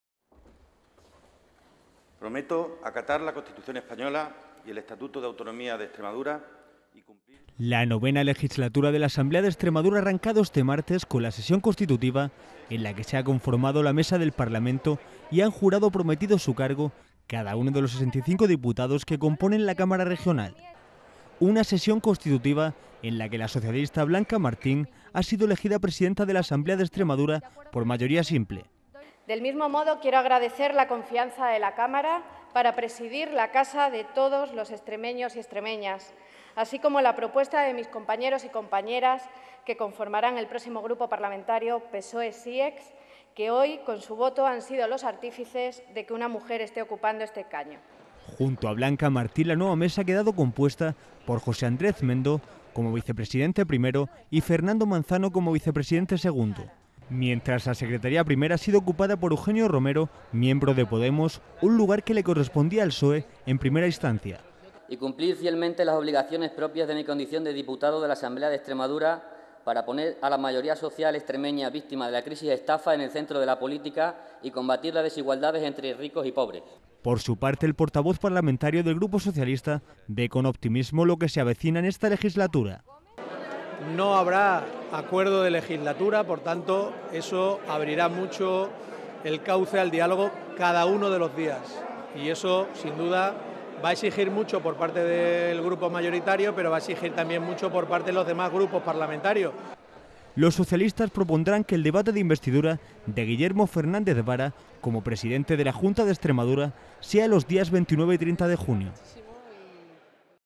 Blanca Martín preside la Asamblea de Extremadura
La novena legislatura de la Asamblea de Extremadura ha arrancado este martes, día 23, con la sesión constitutiva de la institución, en la que se ha conformado la Mesa del Parlamento, y han jurado o prometido su cargo cada uno de los 65 diputados que componen la cámara regional.